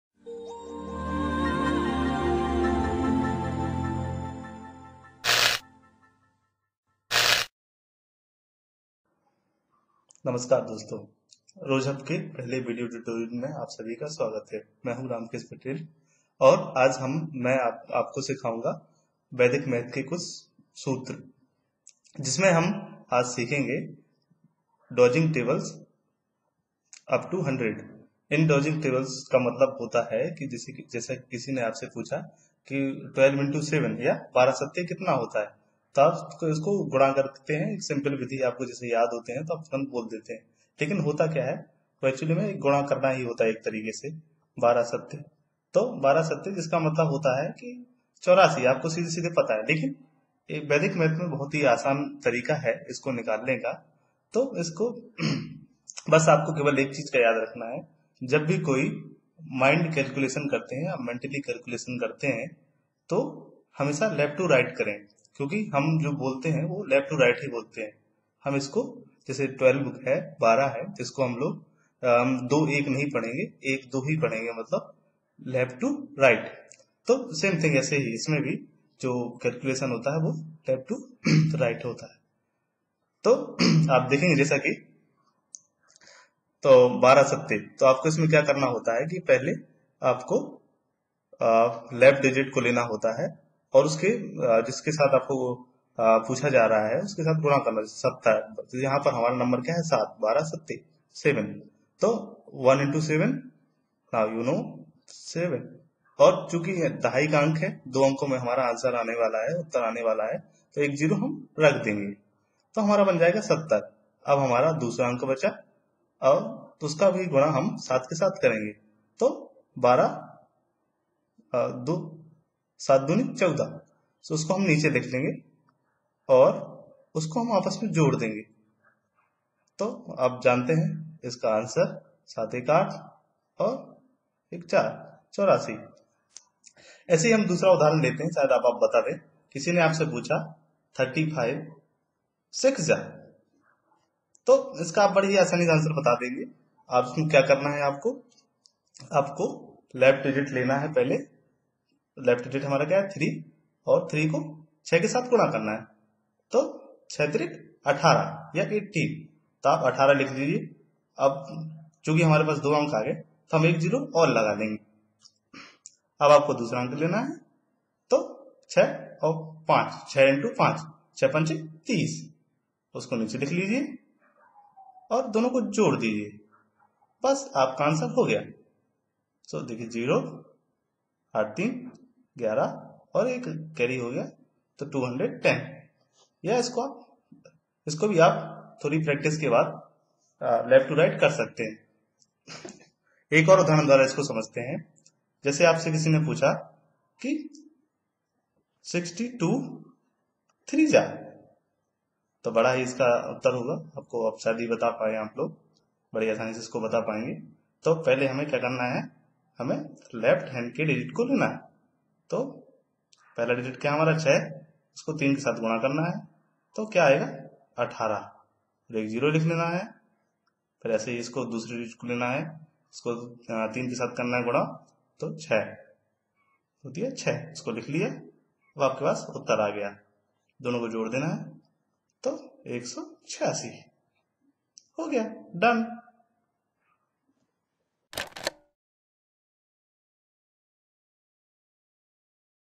Vedic Math learn table up to 100 in a minute | याद करे 1 से 100 तक पहाड़े बस एक मिनट में
By using vedic math you can do fast calculation.
Watch new and easy way learn table in New version of this Video